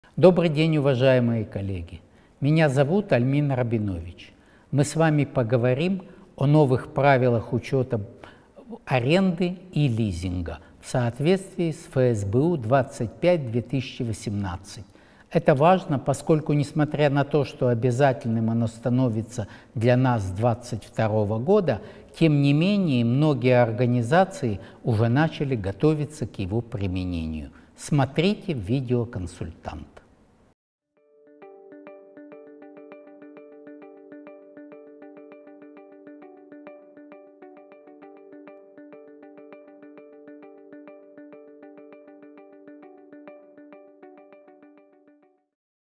Смотрите на Видео.Консультант семинар «Учет аренды и лизинга по правилам ФСБУ 25/2018»
Лектор расскажет, чем новые правила учета аренды и лизинга отличаются от прежних. Вы узнаете, когда надо начать применять эти правила и как на них перейти.
Лектор – Рабинович Альмин Моисеевич, руководитель управления бухгалтерского и налогового консалтинга АКГ «Финэкспертиза», кандидат исторических наук, аудитор.